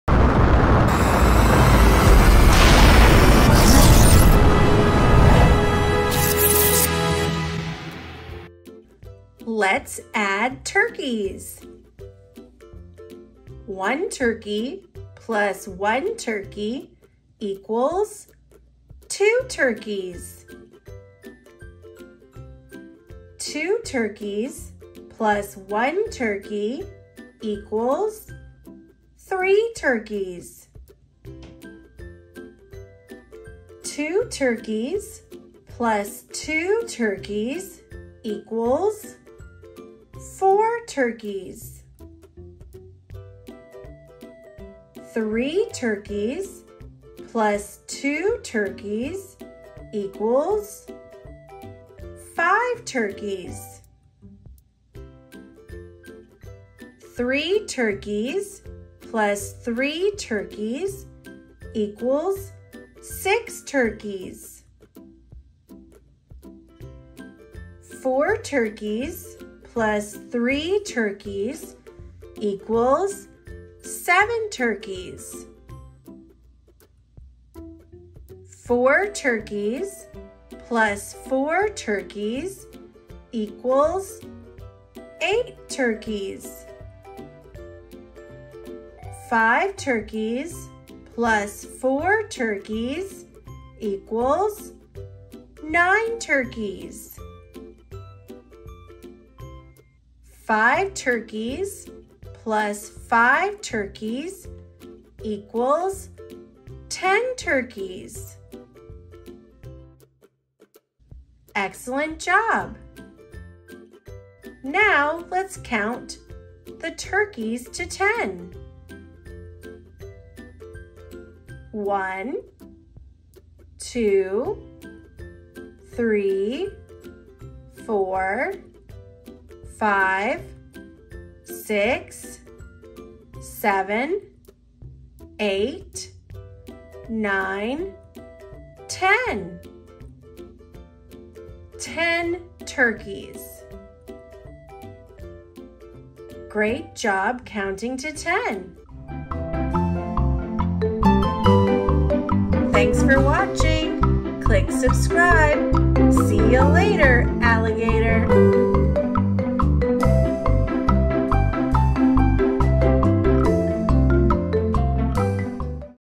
Adding Turkeys Math Fun Animated Pictures Easy to Learn for Toddler/Kindergarten
Visual Learning Easy Math (Adding) for Toddler/Kindergarten
Kids Read Aloud - American English | Kids Story Picture Books (Read/Listen & Learn)